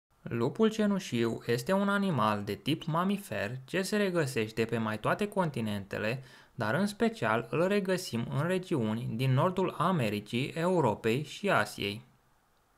Lupul Cenușiu
Povestea Lupului Cenușiu - Învățăm și cunoaștem Lumea Animalelor

Lupul Cenușiu este un animal de tip mamifer ce se regăseste pe mai toate continentele dar în special îl regăsim în regiuni din nordul Americii, Europei și Asiei.  Lupii trăiesc împreună în haite iar dimensiunea unui astfel de lup este la fel de mare ca a unui ciobănesc german. Împerecherea are loc in lunile februarie-martie, dupa care lupul rămane cu lupoaica, pentru a crește puii împreună.
O lupoaică poate făta de la doi până la 6 pui o dată pe care îi alăpteaza vreme de 6 săptămâni. Puii de lup, numiți căței, cresc într-o famile numită haită și au câțiva frați și surori. Un pui de lup cere de mâncare lingând botul mamei sau al tatălui.
Desi nu este cel mai mare carnivor acesta se hrănește cu aproape orice vietate: de la iepuri, mistreți, elani,
căprioare, bizoni și alte animale mari.